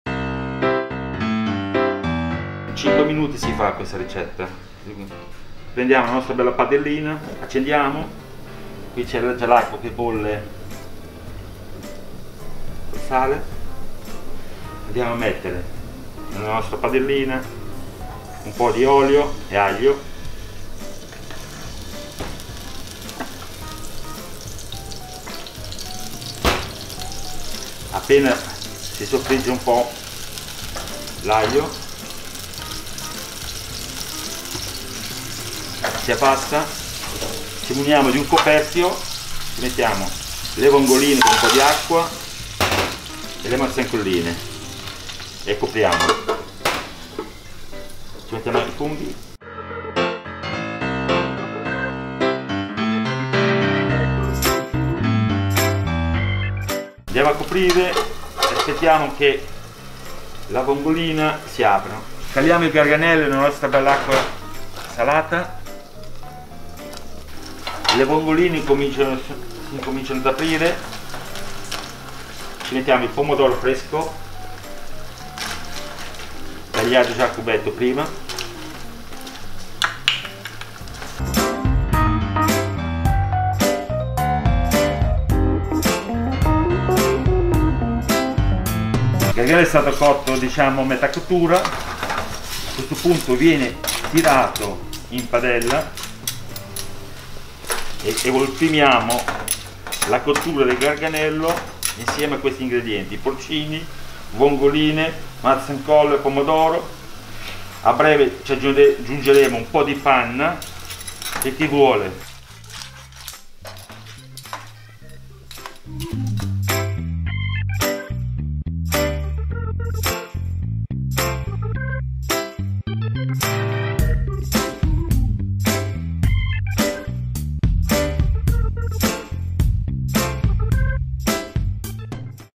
video garganelli mare e monti
Lezione di cucina! Oggi si impara a cucinare un piatto tipico della tradizione marinara. Ecco un video che illustra tutti i passaggi spiegati da un esperto: l’ideale per cimentarsi nella ricetta e nella preparazione di una nuova pietanza. Non importa quale sia la vostra competenza in materia, l’importante è aver voglia di scoprire un gustosissimo tesoro proveniente dal mare.